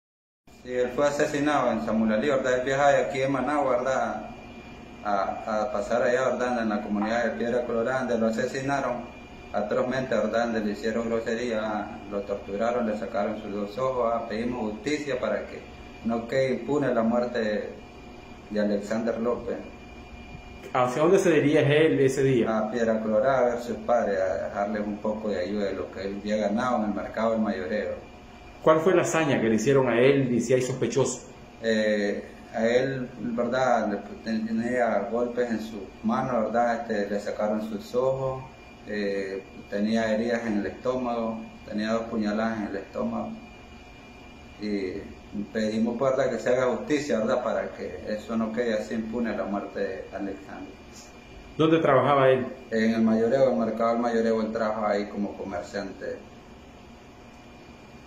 Jairo López relata cómo asesinaron a su sobrino Alexander López por robarle en el Mayoreo
Alexander López de 19 años fue asesinado de dos estocadas en el estómago, luego que tres delincuentes lo interceptaran, para despojarlo de un celular y su salario que había ganado como ayudante en uno de los tramos del Mercado El Mayoreo de Managua

💻 Noticias de Nicaragua | Noticias de Nicaragua de hoy